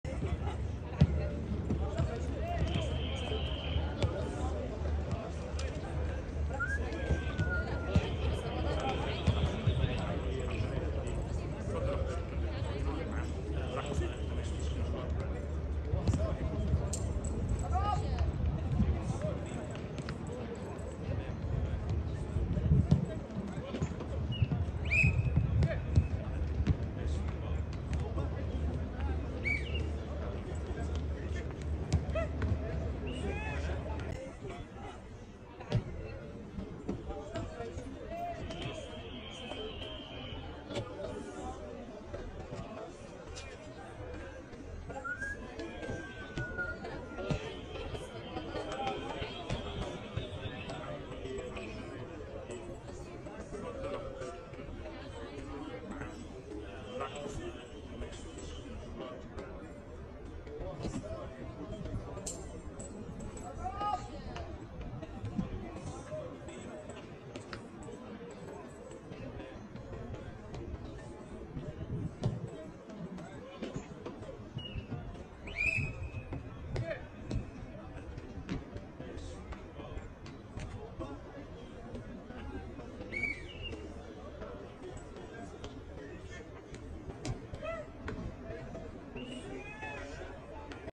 شاهد عبد الله السعيد يسجل هدف عالمي في مرمى شريف إكرامي ويصفق له على روعته
نستعرض في هذا الفيديو هدف عالمي سجله عبد الله السعيد في مرمى شريف خلال احد تدريبات بيراميدز، ومن روعة الهدف صفق شريف اكرامي لعبد الله السعيد

‏Captin.Malouda

‏Captin.Malouda

رمضان صبحي
عبد الله السعيد
شريف اكرامي
بيراميدز